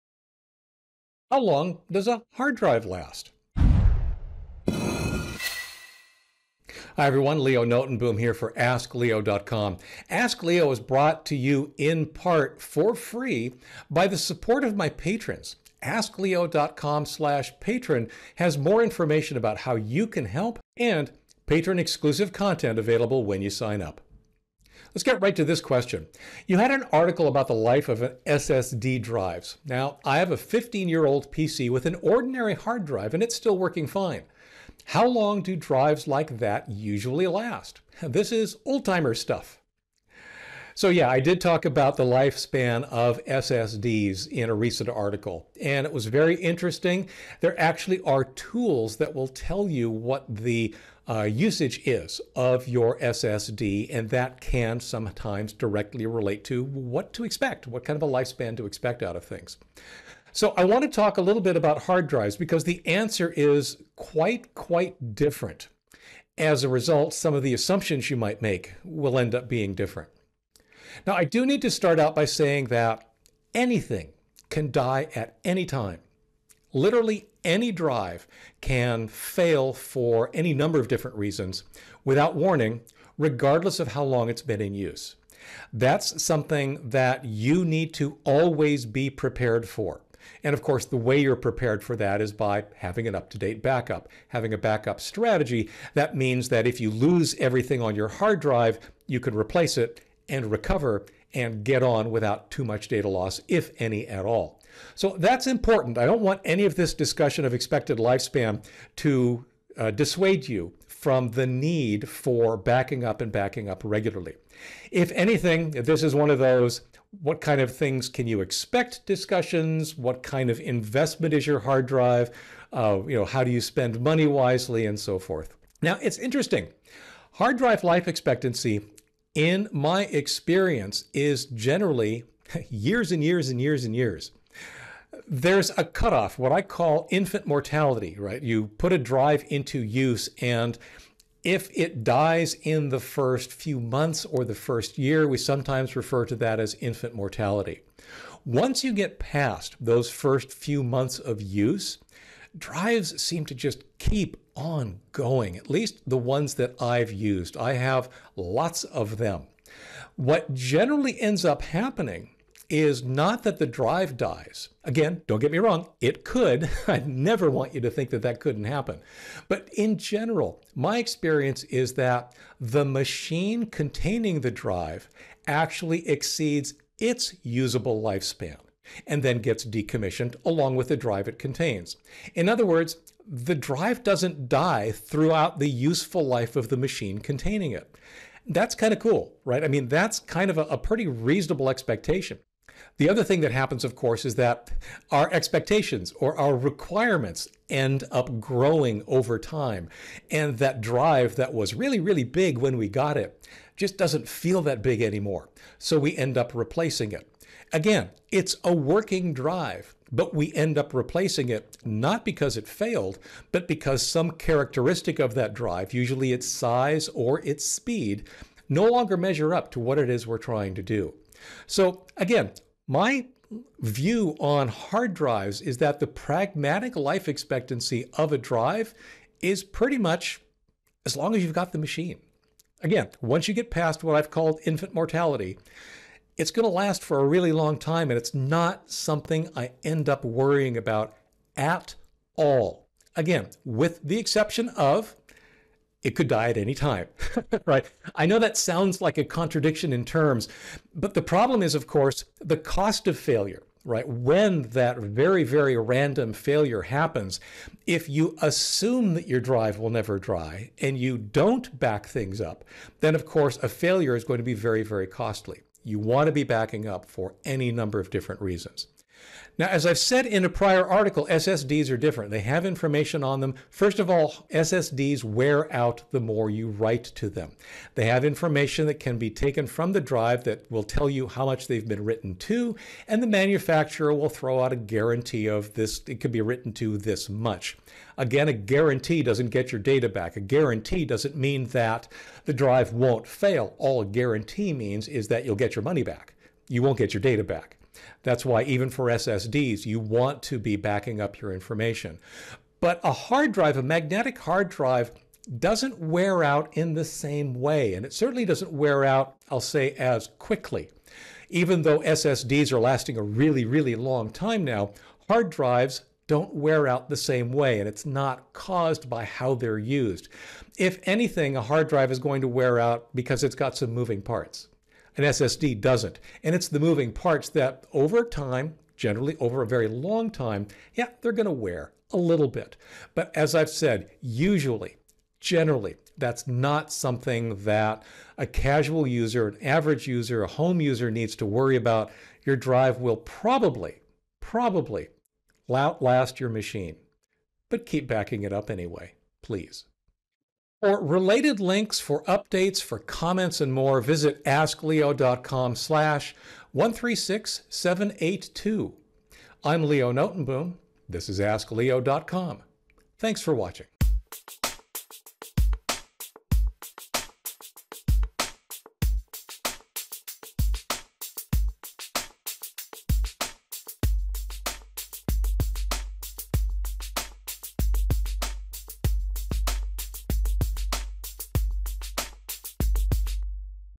How Long Does a Hard Drive Last?
⏱️ A hard drive can last a long time, but it's important to realize that they could still fail at any moment.

HDD lifespan
Traditional spinning magnetic-platter hard drives can last for many years, as reported in data center studies. While failure rates do increase over time, they remain relatively low during the useful life of the computer containing the drive. Regardless of the expected lifespan, it’s critical to assume it’ll fail at any time and be prepared with backups, so as not to lose any data.

Chapters
0:00 How Long Does a Hard Drive Last?
1:10 Anything can die at any time
2:04 HDD life expectancy
3:12 Pragmatic life expectancy
4:30 SSDs are different